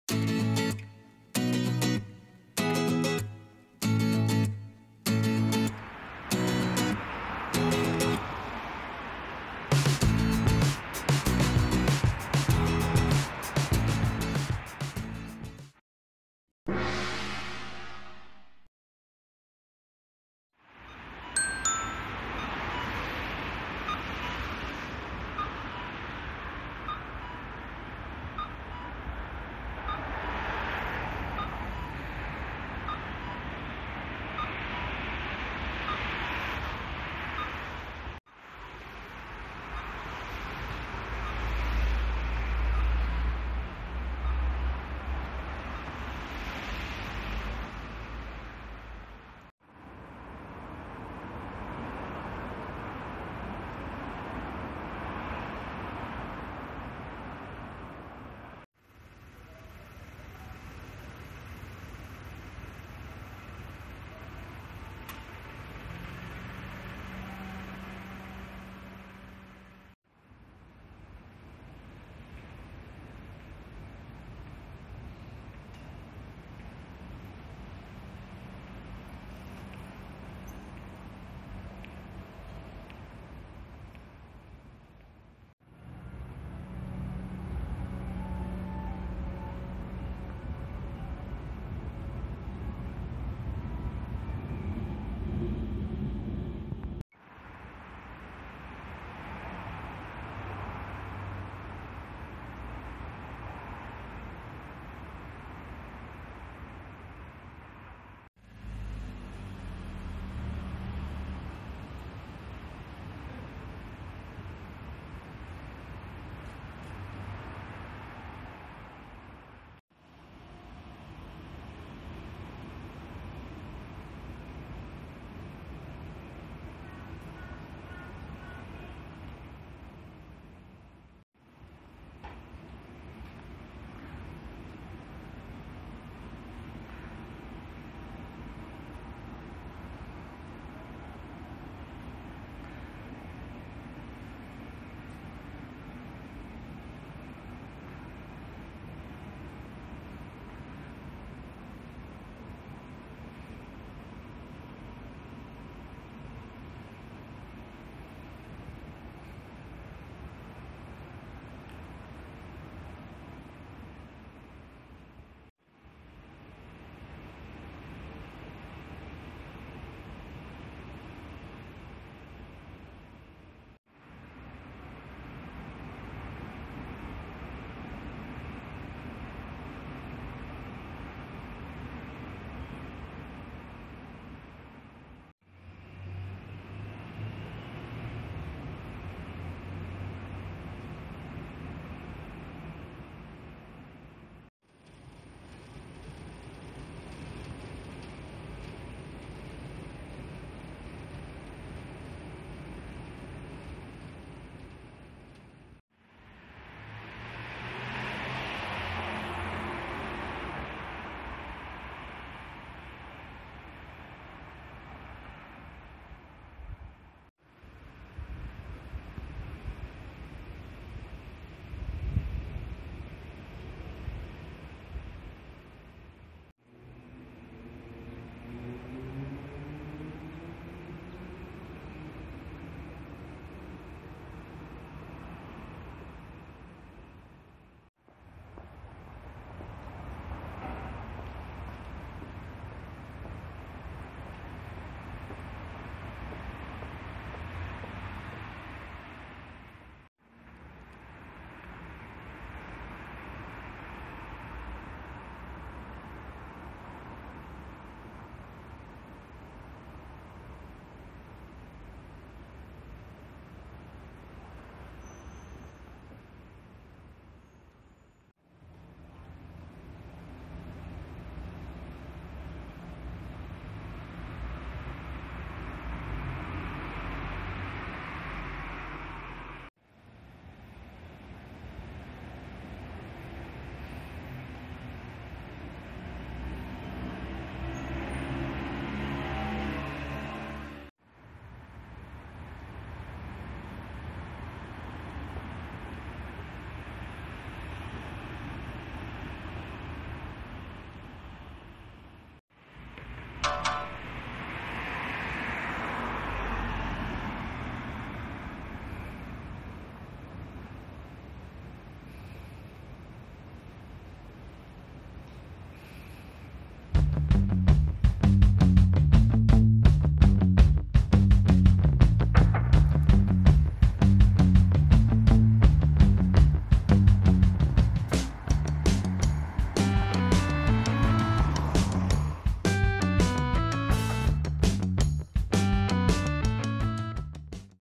大都会に密かに佇むレトロ区画【名古屋市】長者町繊維街を歩く【Downtown Japan】
大都会に密かに佇むレトロ区画【名古屋市】長者町繊維街を歩く

こんにちは！
私は普段日本の下町を撮影しています。この動画を広めてくれたら嬉しいです！